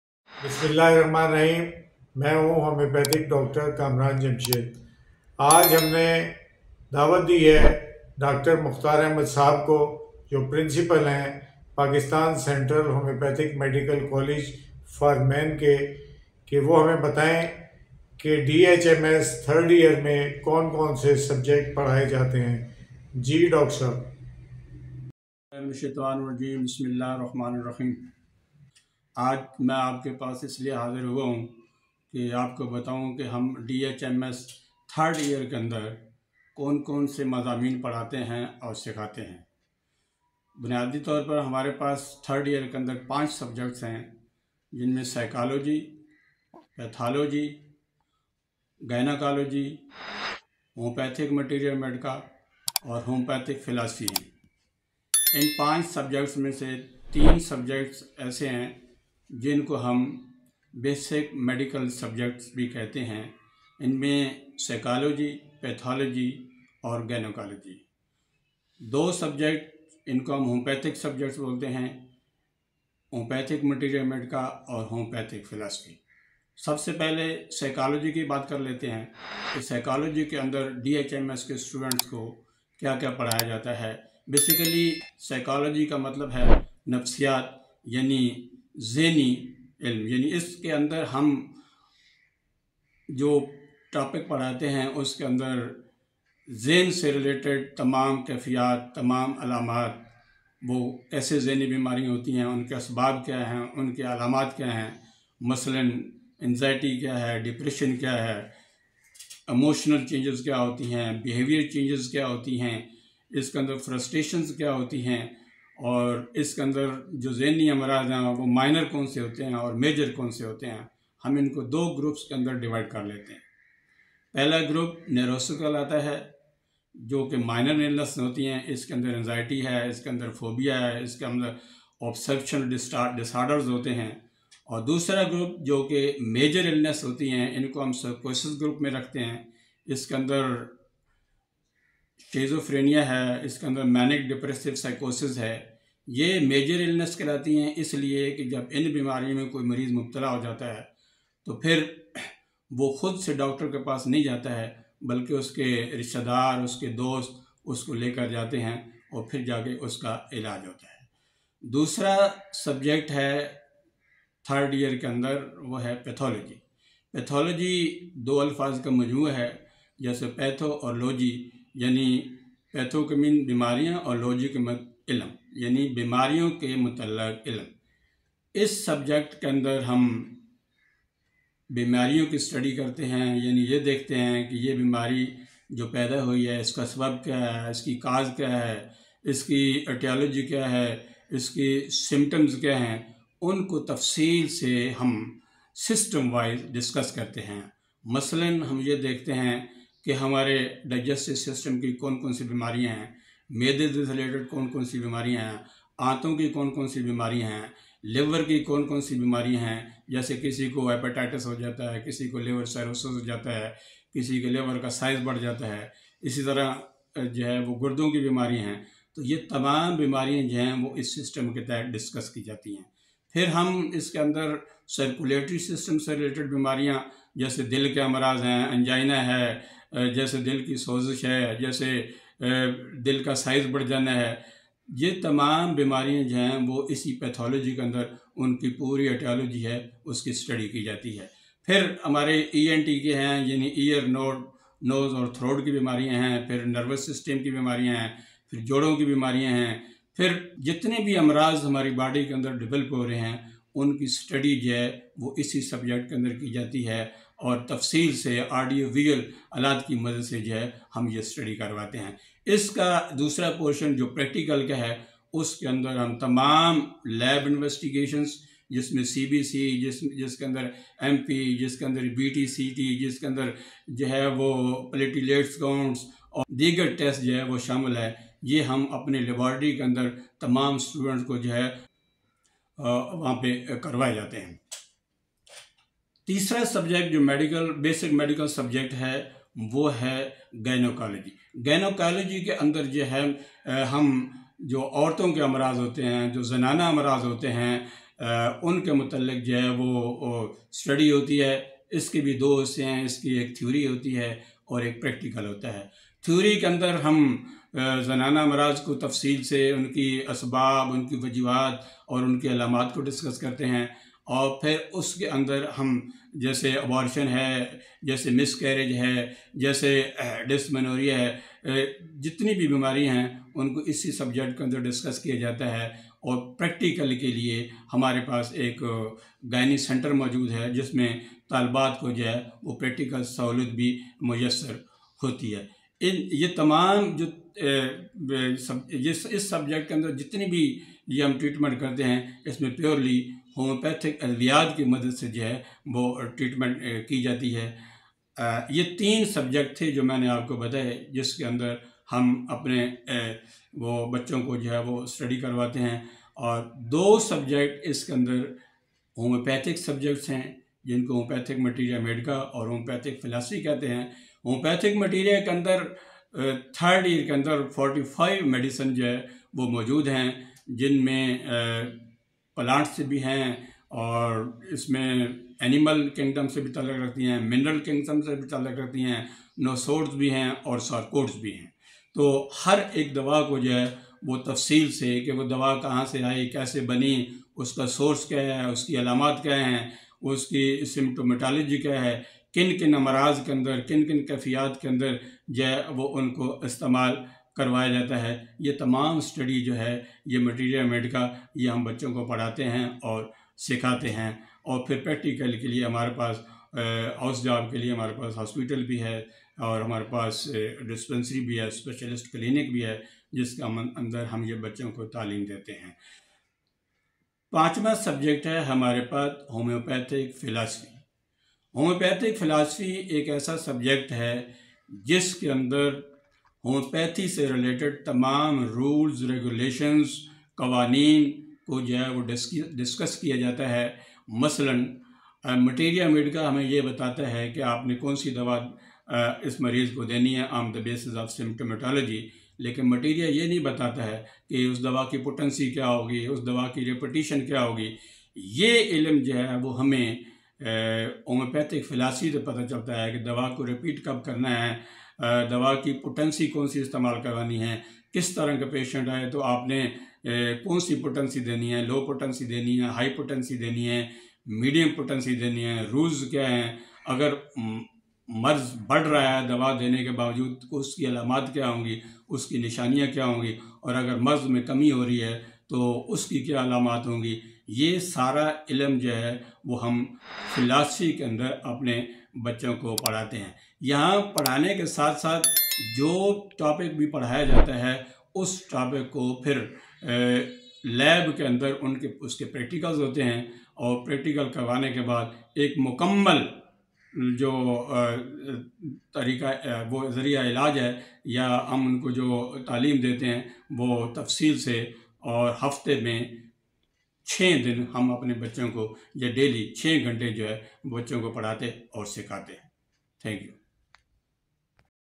Subjects of DHMS third year by Dr kamran jamshed
Well explained subjects of DHMS for guidance of students you can become Dr Homeopathic